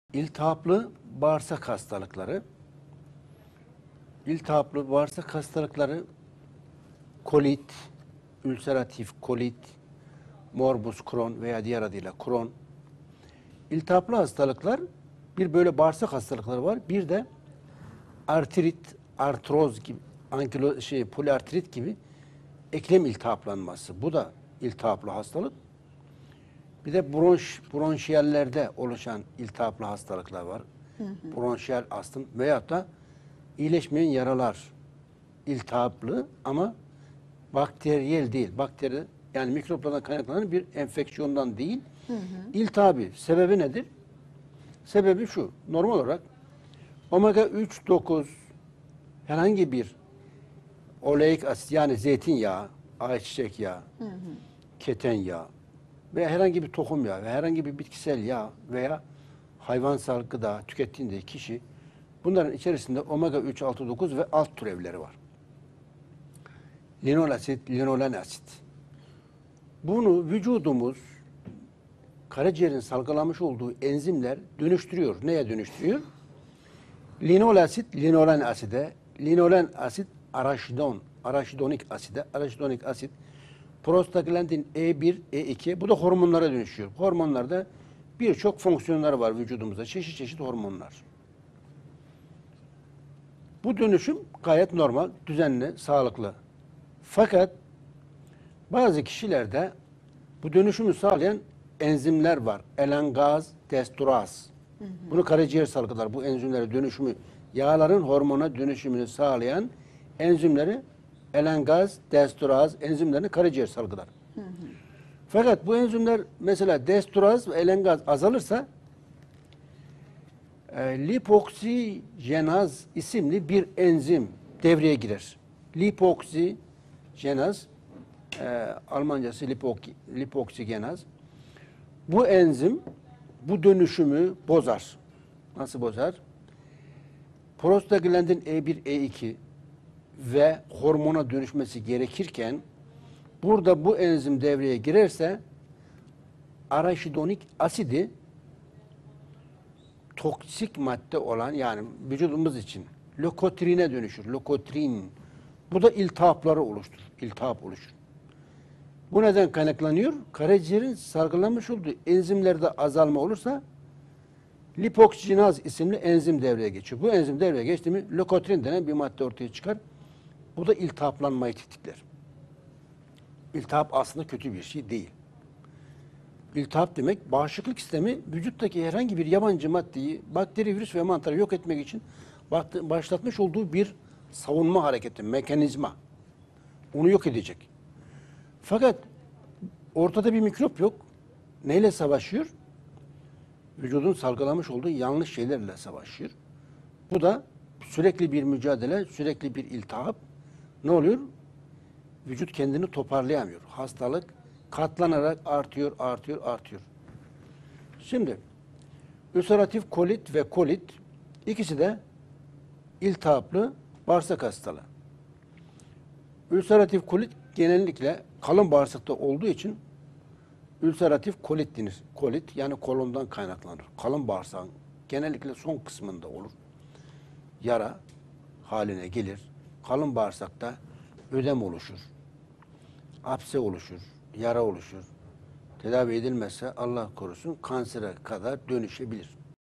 İltihaplı Bağırsak Hastalıkları, Ülseratif Kolit Tedavisi, Kron Hastalığı
Bitki Bilimci ve Araştırmacı Yazar İbrahim Gökçek iltihaplı Bağırsak Hastalıkları, Ülseratif Kolit Tedavisi, Kron Hastalığı, Kron Tedavisi, Morbus Kron Tedavisi, Ülseratif Kolit Tedavi, iltihaplı Bağırsak Hastalığının Tedavisi, Bitkisel Tedavi ile ilgili soruları cevaplıyor.